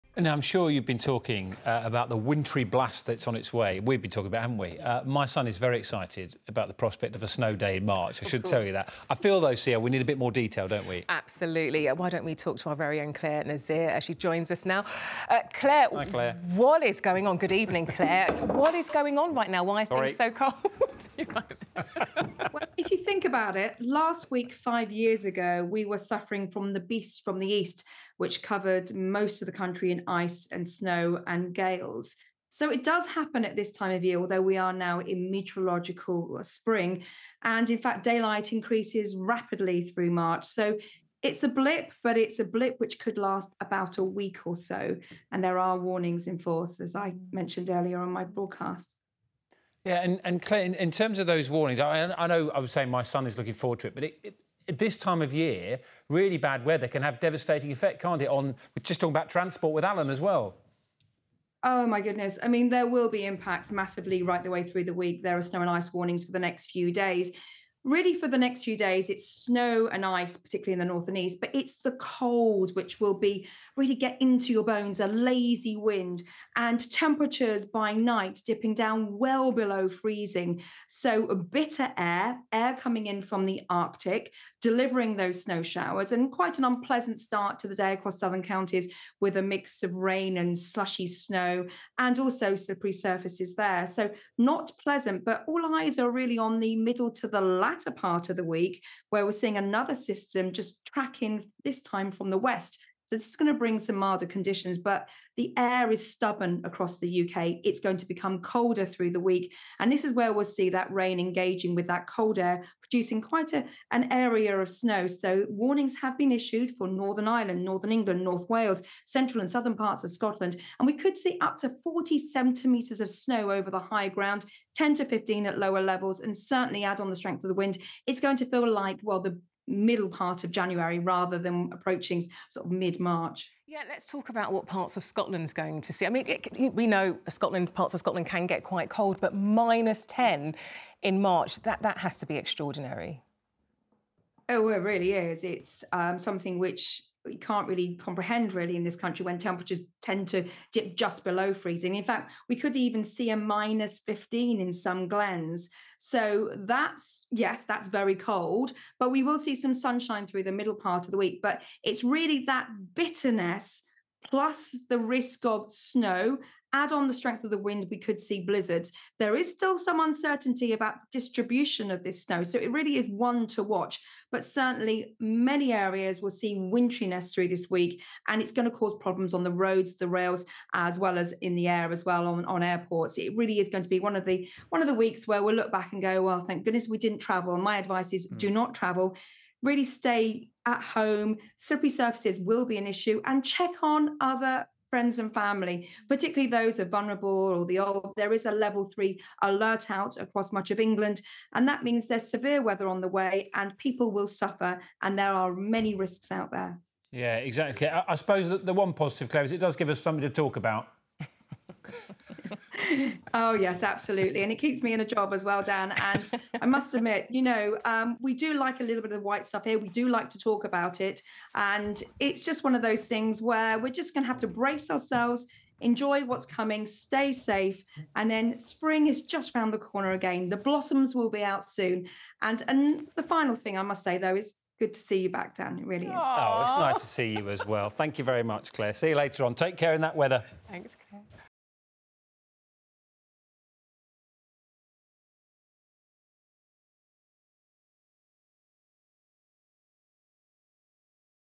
Snow and ice weather warnings issued across the UK | 5 News
► Warnings of snow and ice have been issued by the Met Office as forecasters warn a major change is underway due to Artic air sweeping in from the north.

Yellow warnings will cover much of the UK during the next five days.